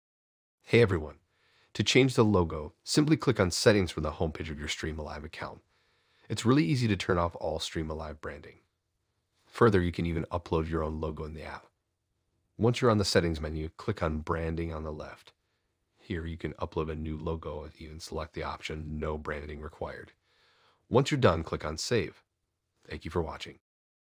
Can you turn off all StreamAlive branding?
Yes! StreamAlive allows you to remove the company's branding as well as the choice to upload your own logo which pops up on the interactions that you are running.

Learn how to make your live sessions come alive with StreamAlive.